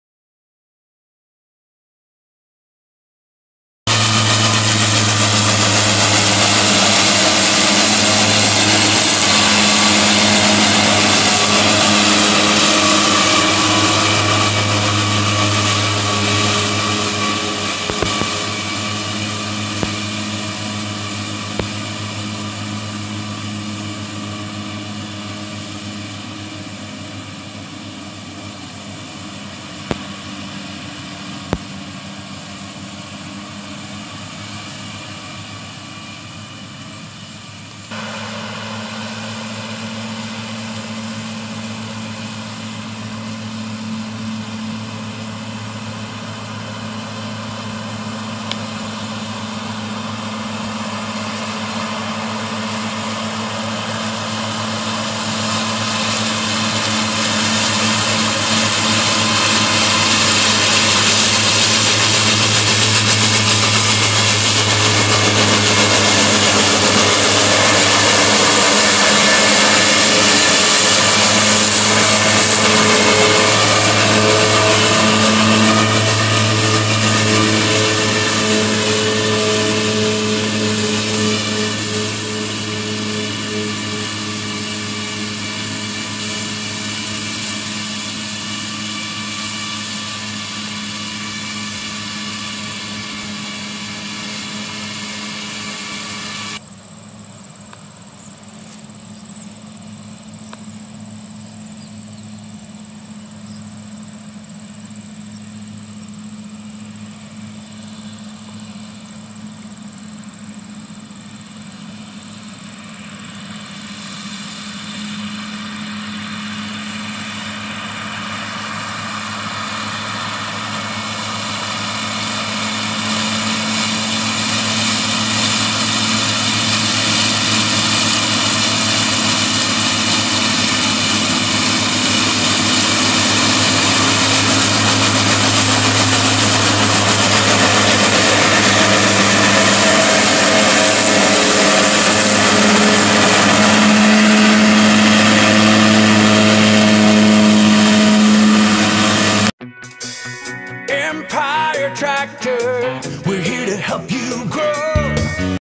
Empire Tractor Presents: SIP In Field Demo
Check out this SIP Mower in action!

September 2018 - Live video footage taken by our Salesman Frank Mutz